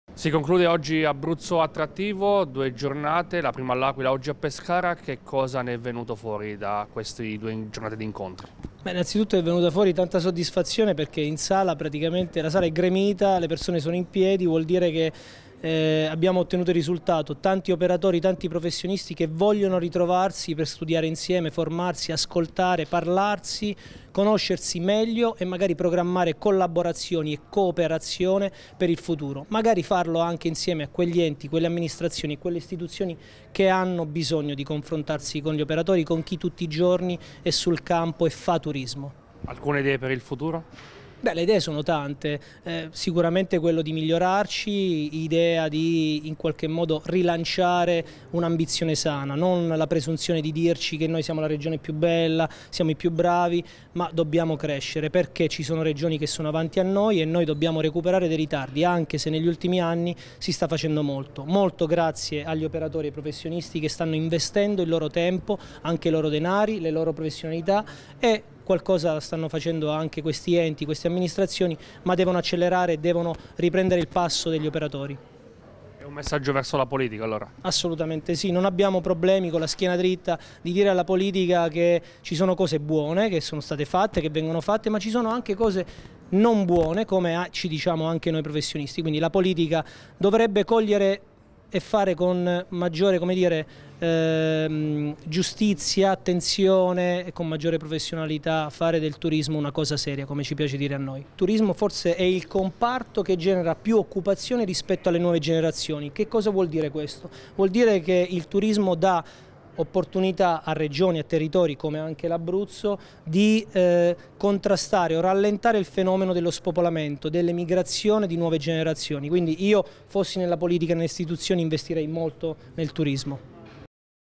Turismo, si chiude oggi Abruzzo Attrattivo
Giornata conclusiva questa mattina a Pescara per la due giorni di Abruzzo Attrattivo. Organizzato da FederPate, la manifestazione, ormai da anni, chiama a raccolta il mondo del turismo abruzzese